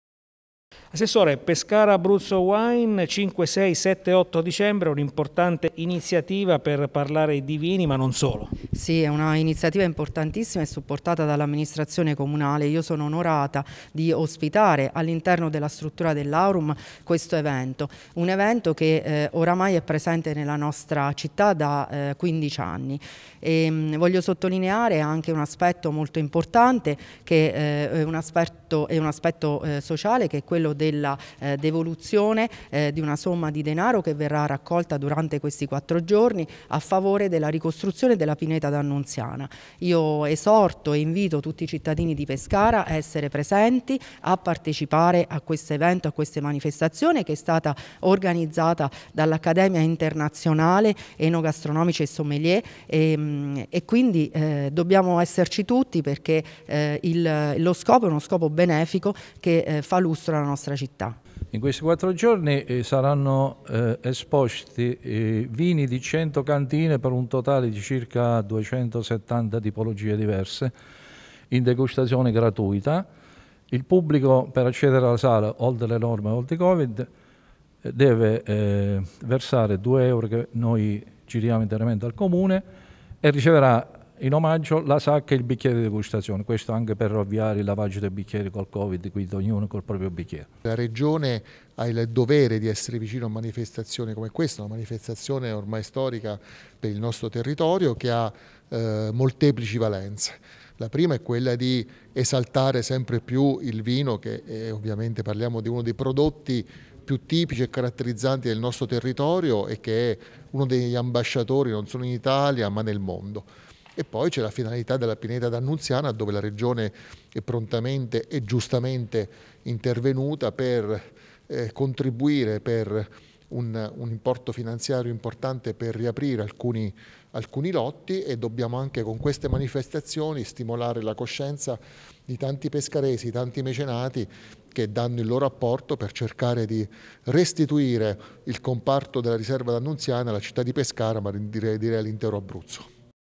Torna “Pescara Abruzzo Wine” giunto alla 15esima edizione
Presentata la 15sima edizione del  "Pescara Abruzzo Wine, Premio Vino e Cultura Città di Pescara" 2021, in calendario all’Aurum nei giorni ‪5-6-7-8‬ dicembre e alla quale hanno garantito la presenza oltre 100 cantine.